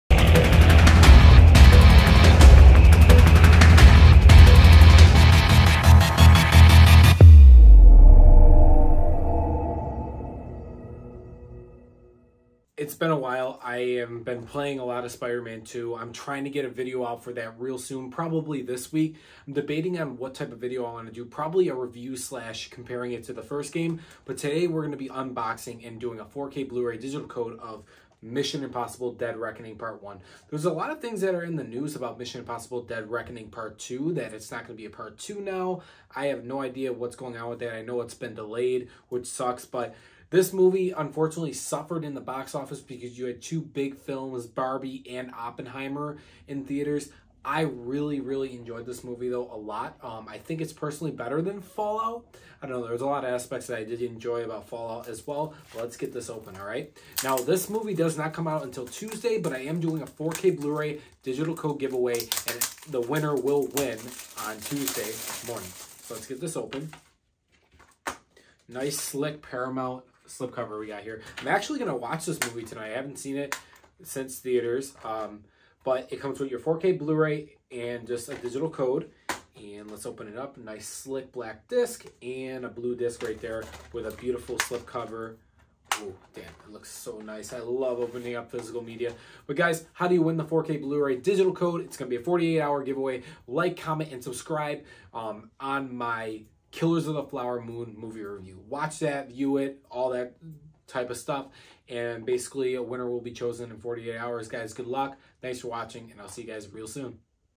Mission Impossible: Dead Reckoning Part 1 4K Blu Ray Unboxing Free Digital Code
4. Winner Chosen In 48 Hours!!